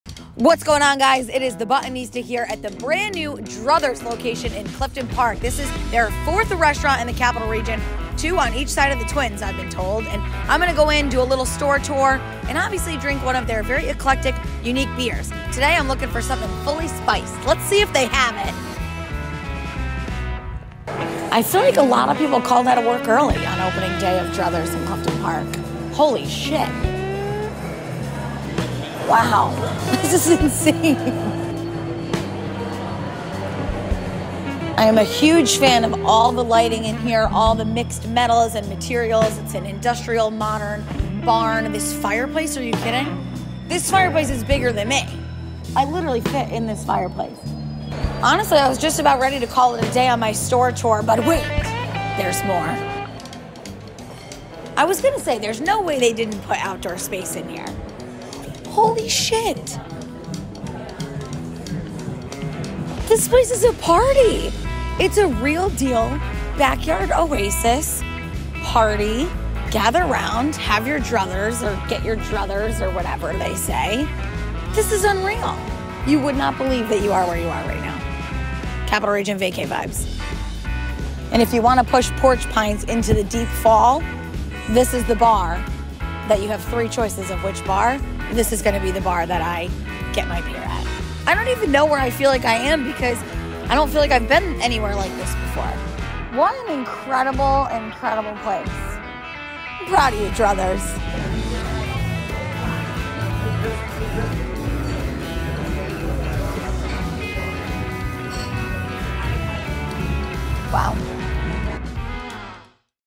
Store Tour: Druthers In Clifton Park
The Buttonista tours the brand new Druthers Brewing location in Clifton Park, NY.